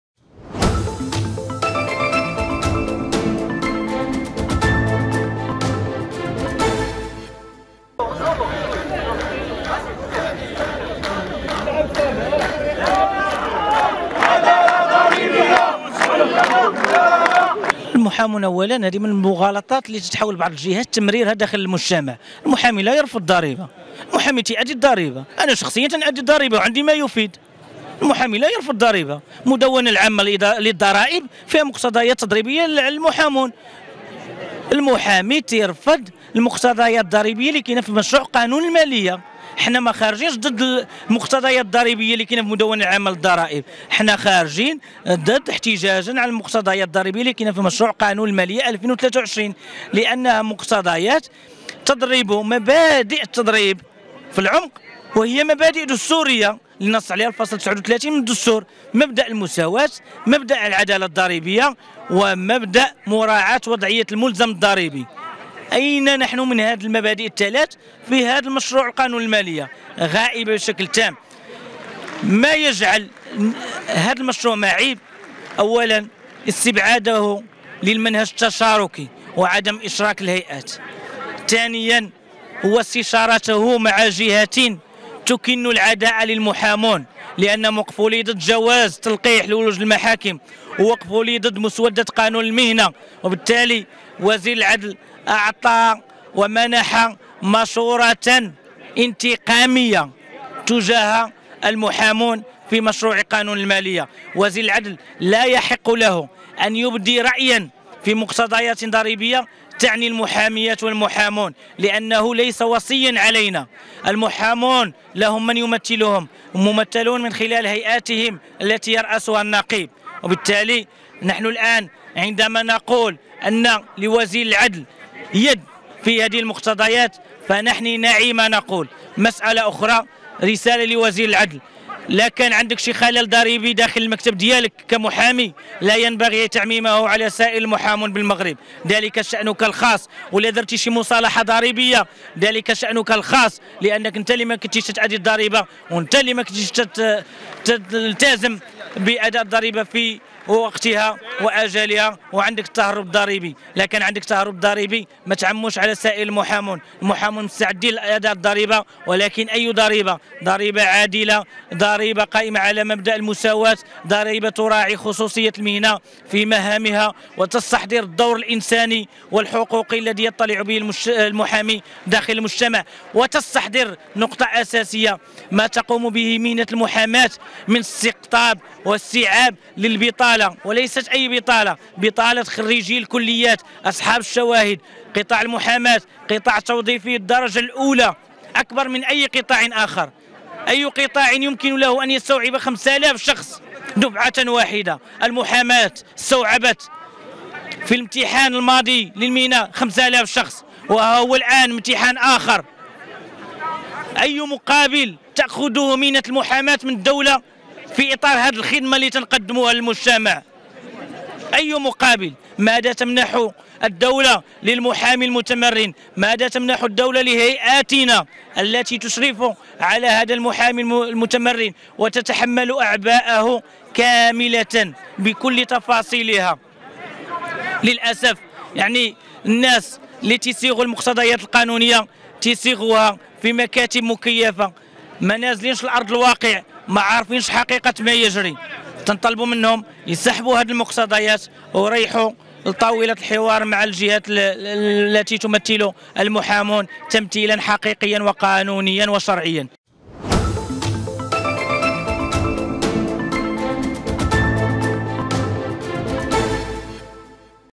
الزرقاوي: المحامون لا يتهربون من أداء الضرائب والوزير وهبي يمارس الإجحاف
نظمت هيئة المحامين بالدار البيضاء على غرار باقي هيئات المحامين بالمغرب، وقفة احتجاجية أمام محاكم الاستئناف، أعلنوا فيها رفضهم لمقتضيات مشروع قانون المالية لسنة 2023، في شقه المتعلق بالضرائب، معتبرين إياه شكلا من أشكال الحيف ضد مهنة تؤدي ادوارا اجتماعية في امتصاص البطالة